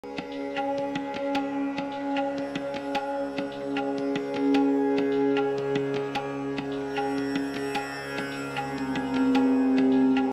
Alien Projections
Seamless loop created for various psychedelic projects and stock footage.